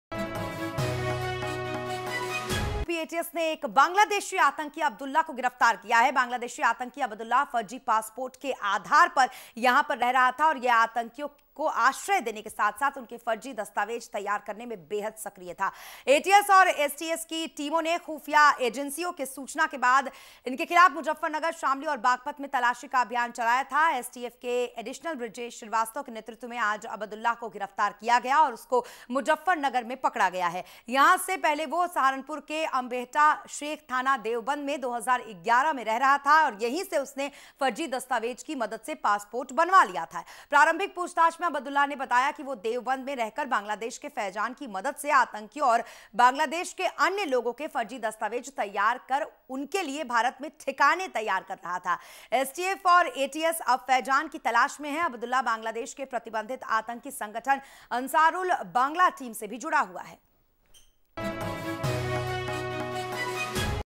UP ATS arrested a Bangladeshi terrorist Abdullah in Muzaffarnagar
Uttar Pradesh ATS arrested a Bangladeshi terrorist Abdullah in Kutesra area of Muzaffarnagar...